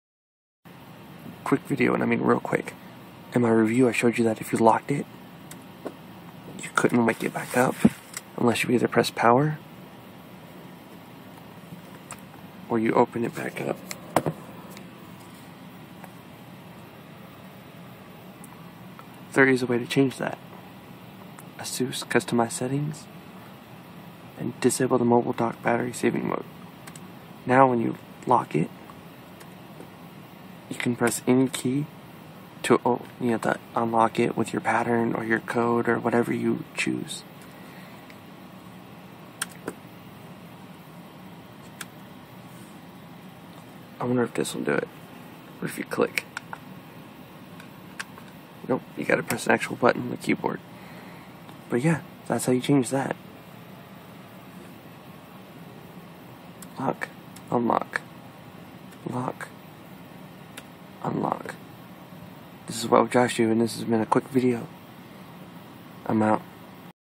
How to wake up the Transformer Prime from the Keyboard Dock
OFFICIAL Android 4.1.1 Jelly Bean!

Really long title! I know, I'm sorry. Do you like these kinda tip videos?

Basically there is a way to unlock the Transformer Prime from the keyboard dock without having to press power, and this video shows you how to do it!

You CAN send your UNLOCKED Transformer Prime in for Repair!
Kwongster94 sent his in and confirmed this!